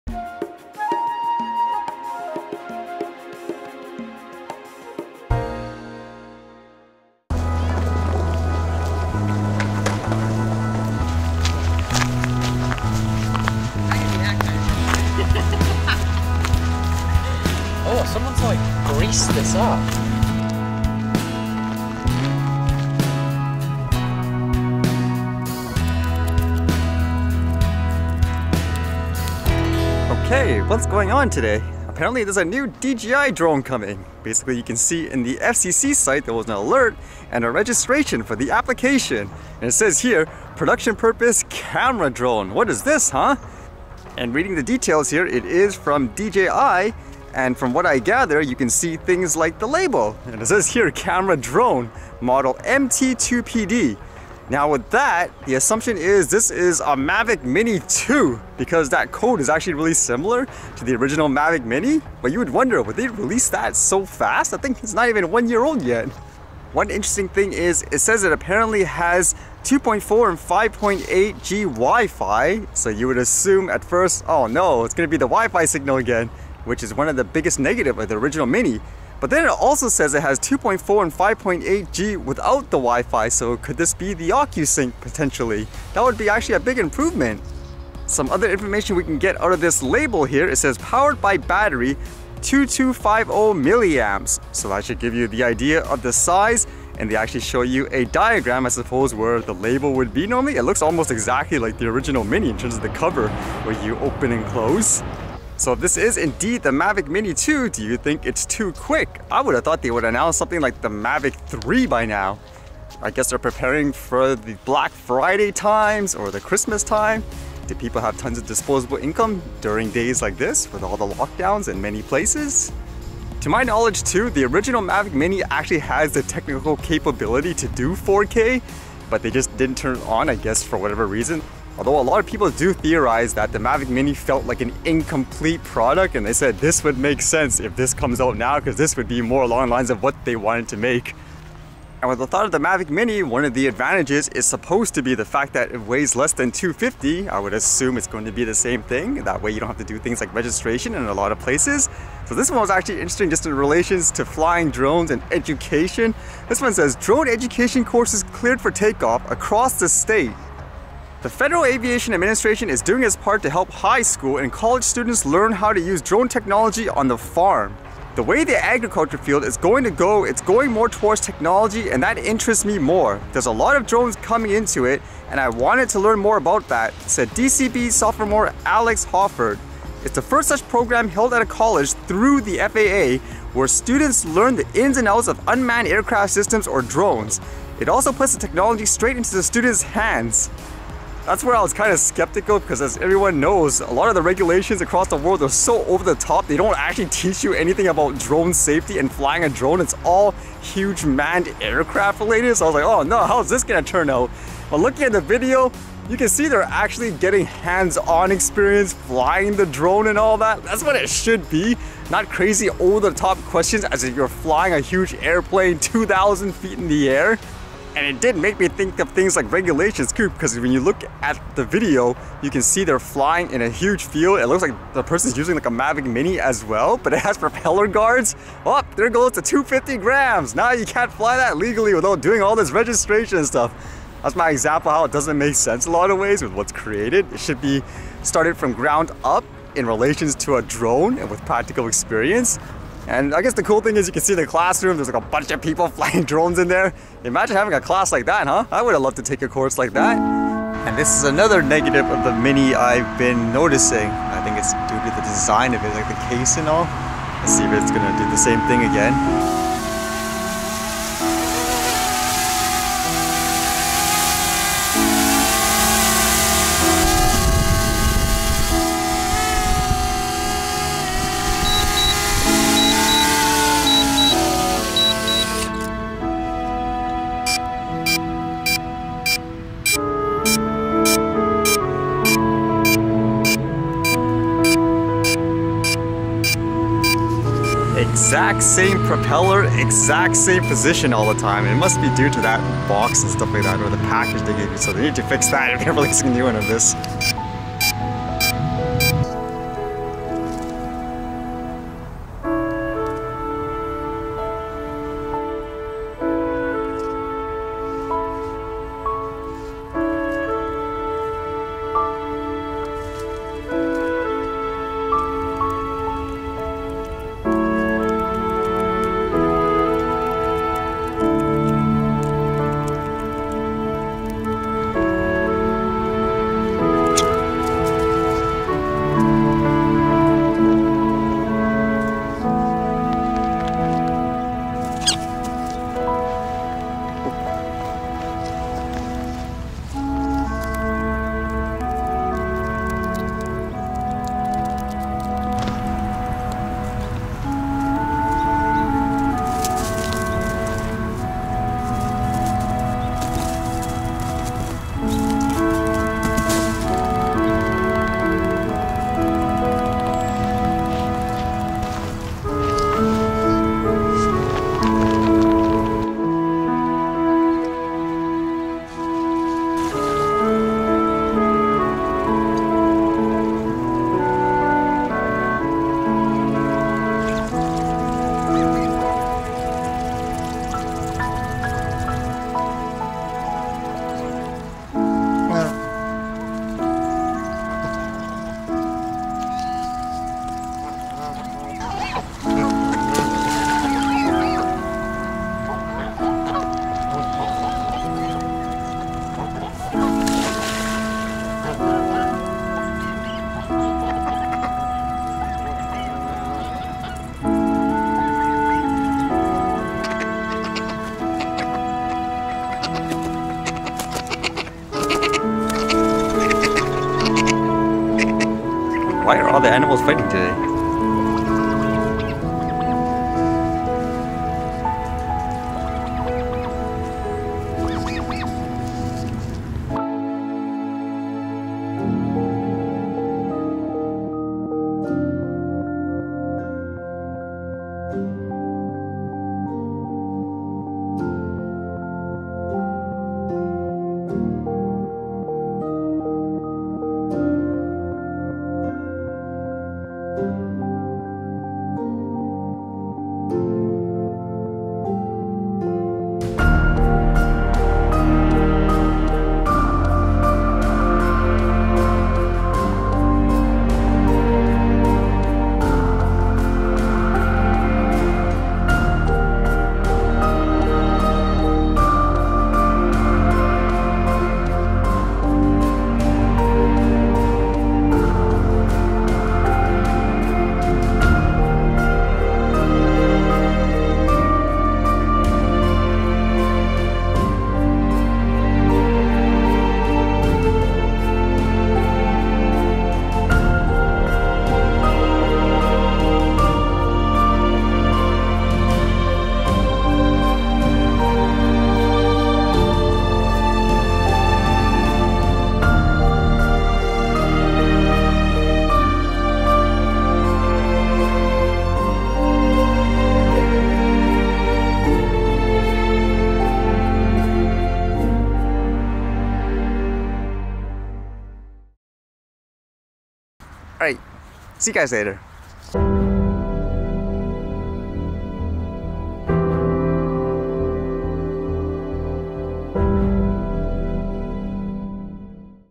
Mavic Mini 2 FCC Details, Public Drone School Education
-Details about a DJI drone camera release by the official FCC site.  Assumption is this is the Mavic Mini 2.
-Schools getting drone education courses through the FAA
-Mavic Mini ESC Error.  ESC Beeping (Code:30128) and Propeller rotating too fast error.
-Animals getting into fights
-Squirrels, ducks, geese
-Thermal imaging video of birds
-Drone video of sunset and sky

0:00 Intro
0:29 Mavic Mini 2 FCC
2:21 Drone Education
4:09 Mavic Mini Beeping Error
4:59 Wildlife Rabbit Thermal
7:11 Drone Video

My Gear and more: